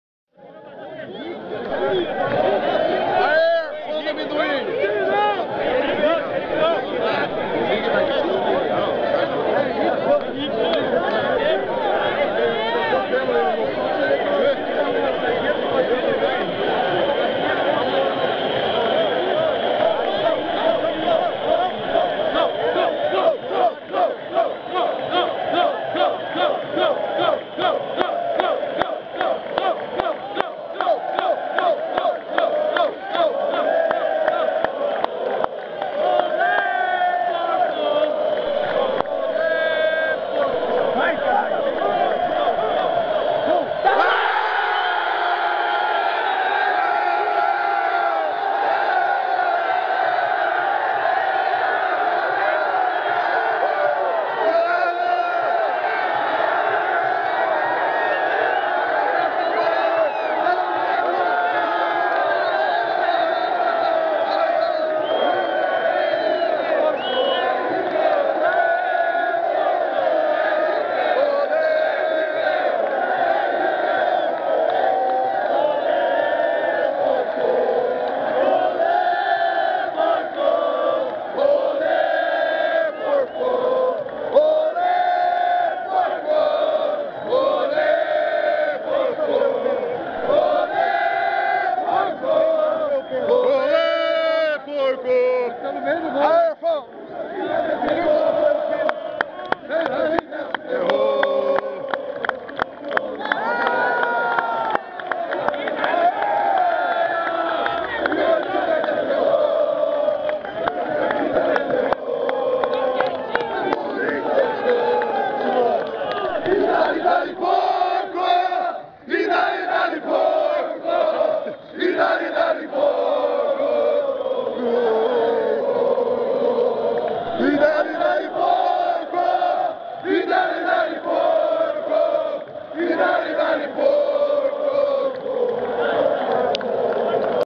Paraná 1 x 2 Palmeiras 25/04/2012 Vila Capanema - Paraná
Vídeos feitos por mim durante o jogo.